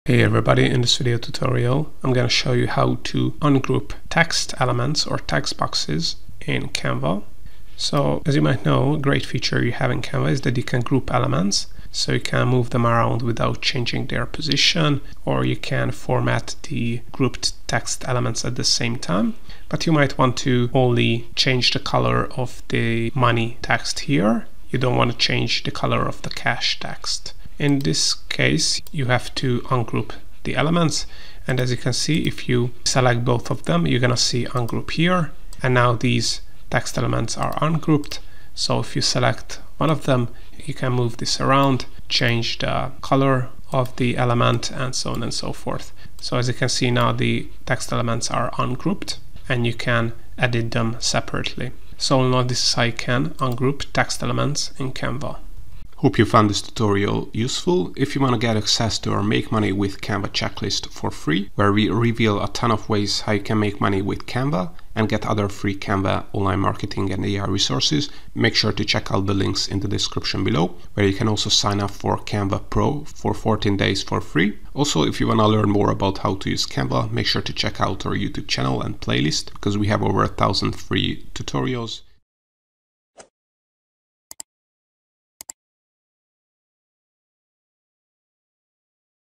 How To Ungroup Text Elements in Canva? [in 2025]
Learn how to ungroup text elements in Canva.